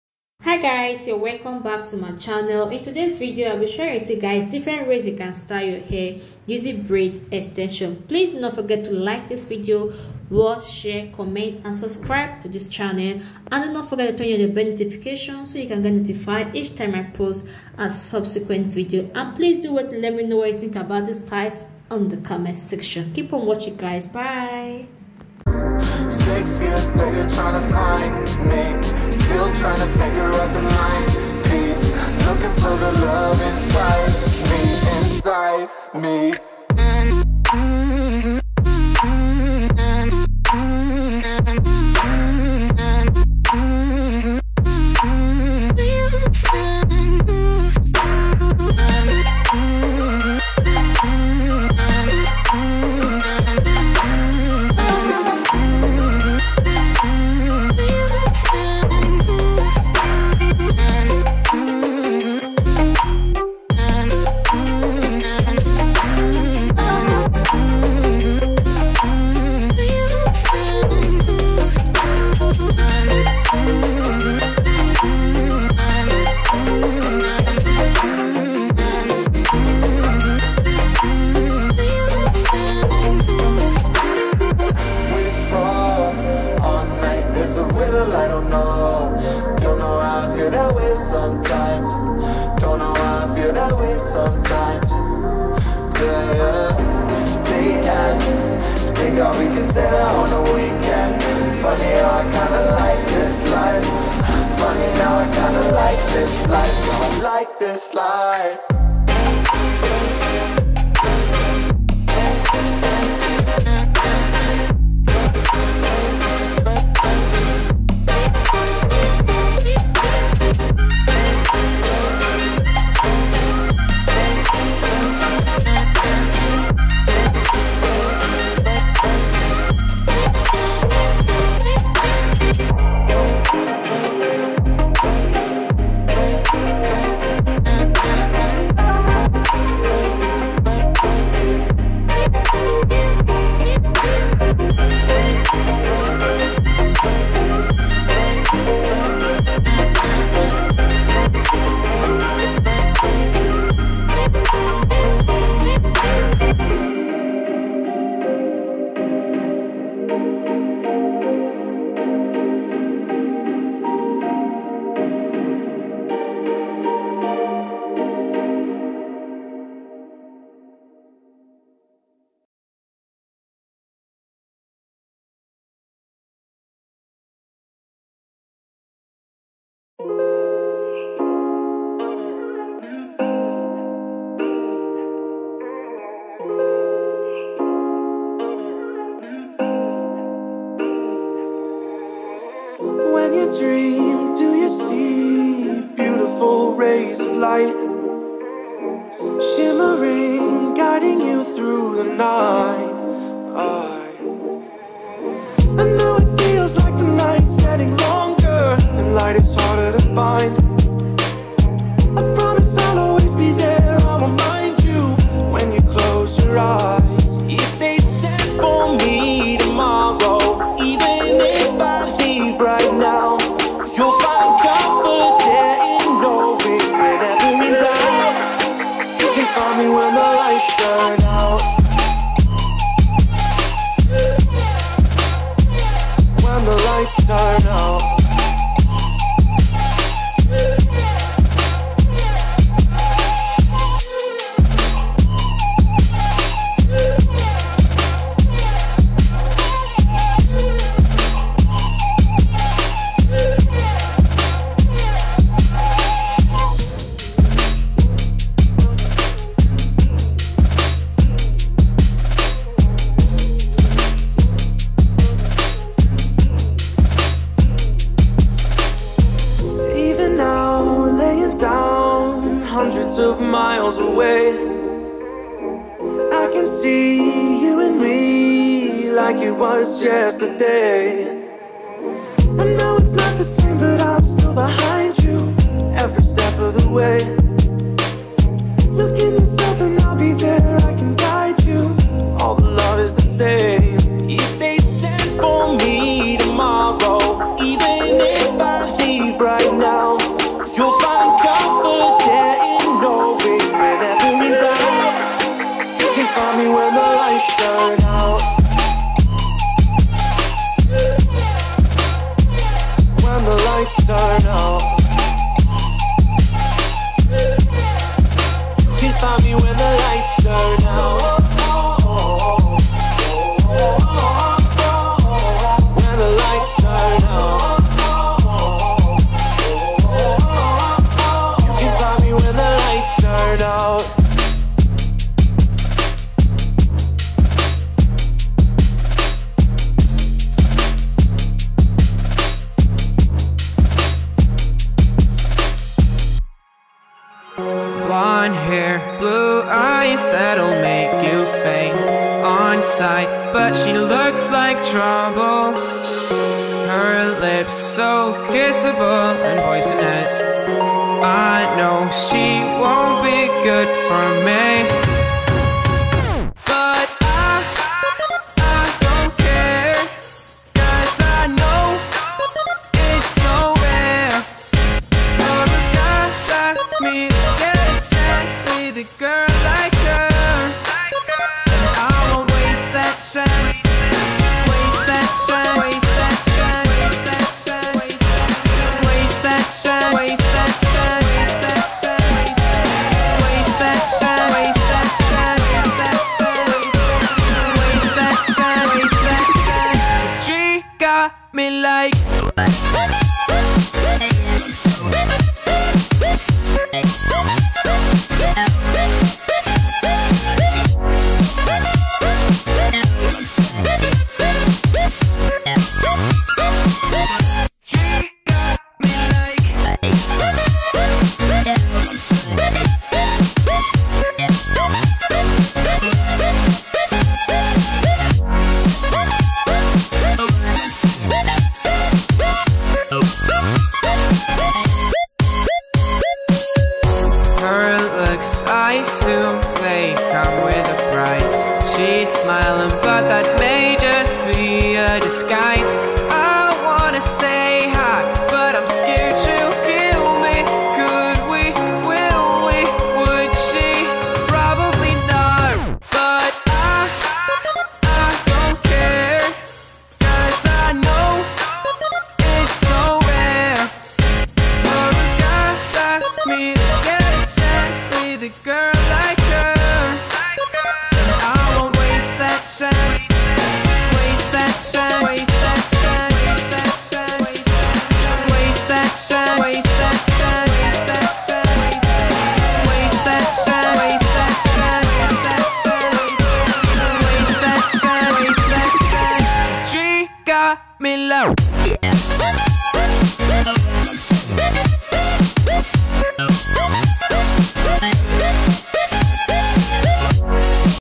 How to style your natural hair using braid extension/beautiful hair style
hello friends you are welcome to my channel.today i will be sharing with you guys different ways you can style your natural hair using braid extension.please watch, like, share, comment and subscribe to this channel. thanks